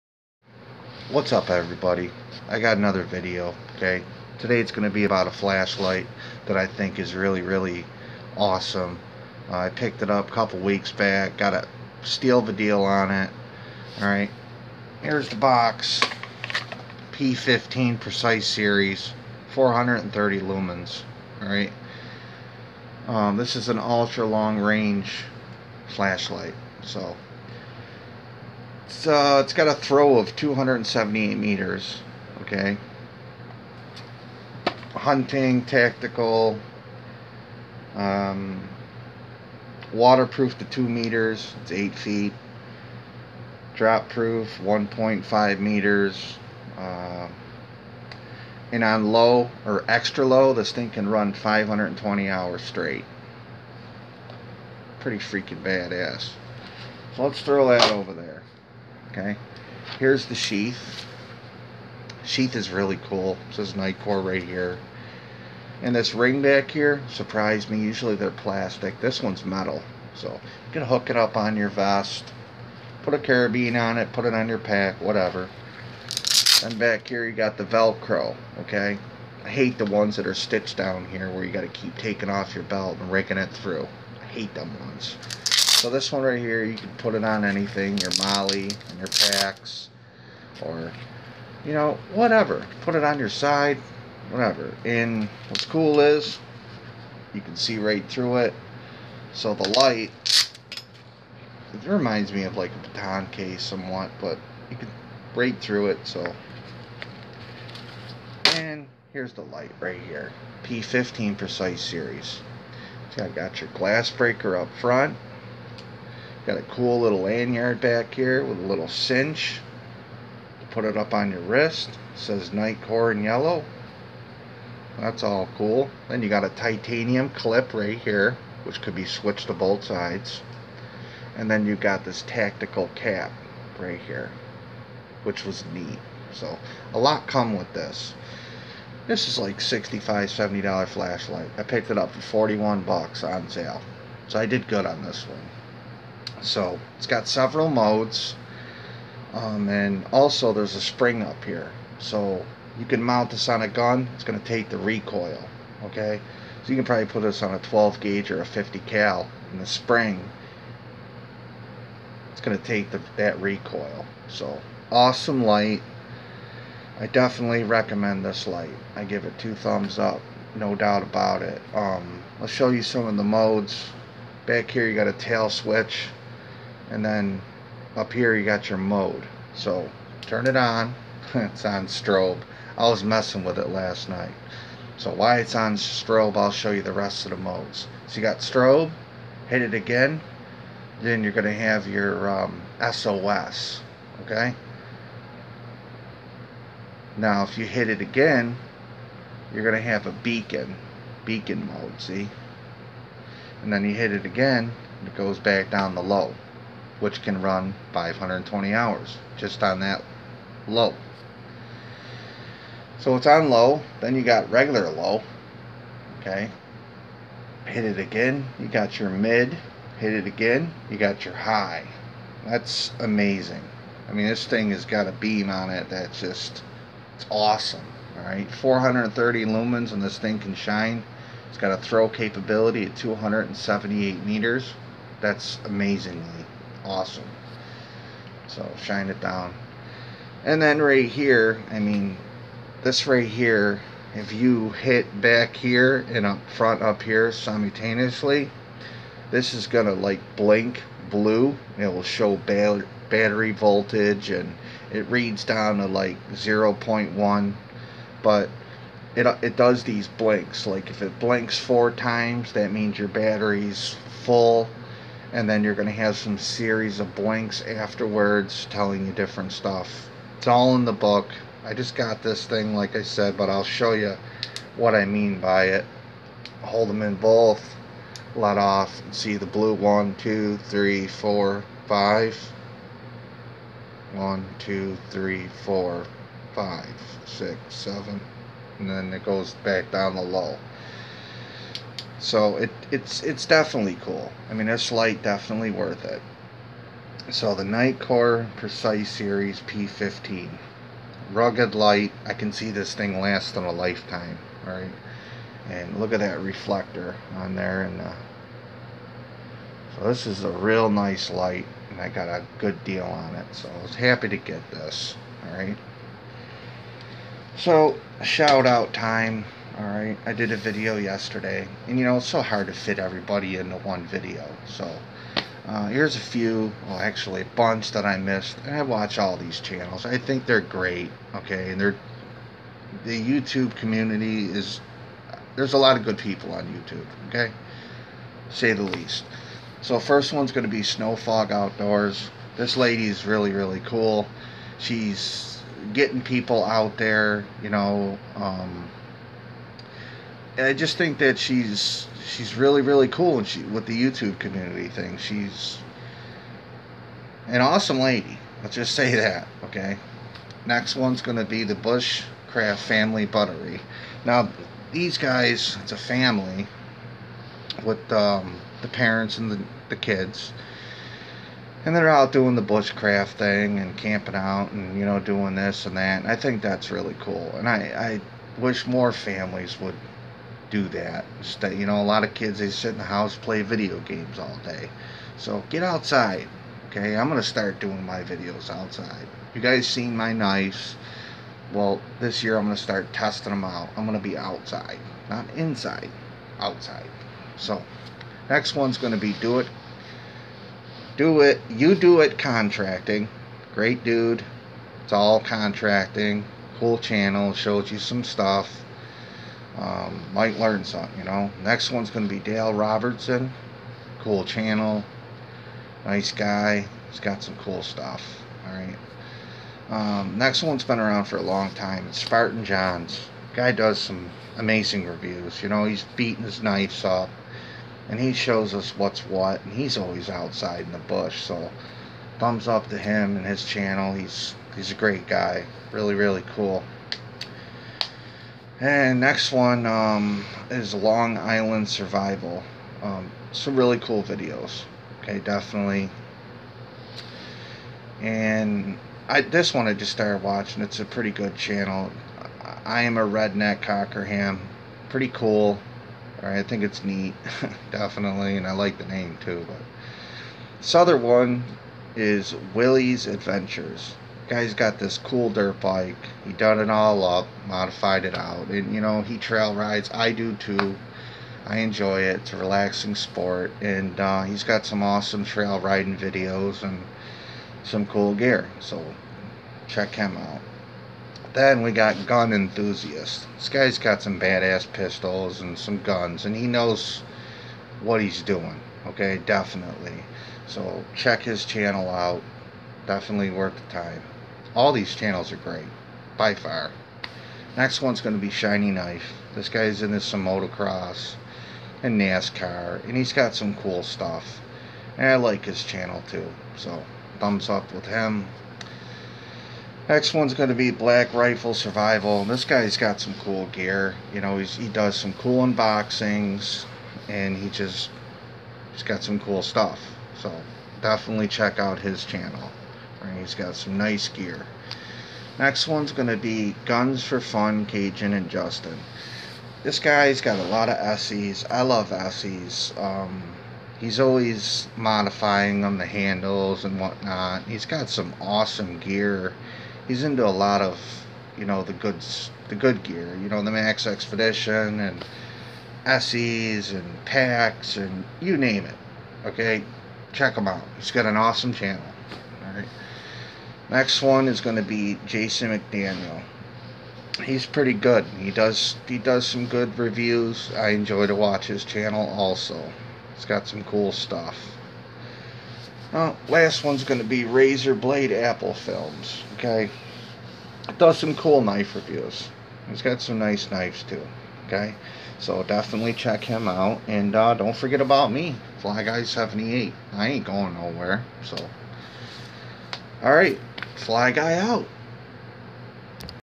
NITECORE P15 precise series 430 lumens +more SHOUT-OUTS!
This flashlight is definitely a really nice light to have, I must say it has impressed me alot so far I'm very happy with my purchase, so get one now !